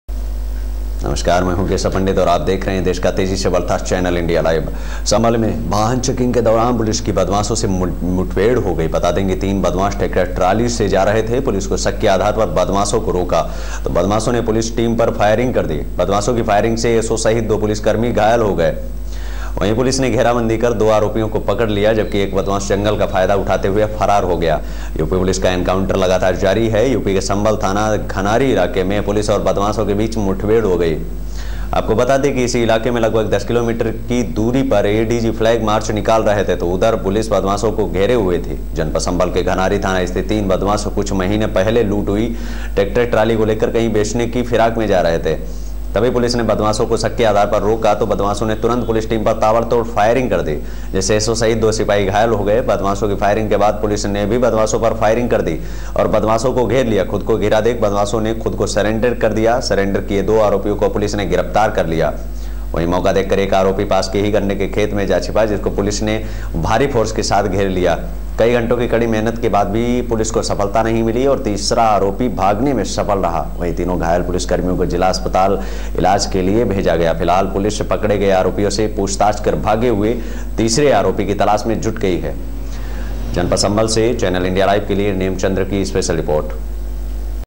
पुलिस और बदमाशों में मुठभेड़ Channel India Live
संभल से चैनल इंडिया लाइव के लिए  नेमचंद्र की रिपोर्ट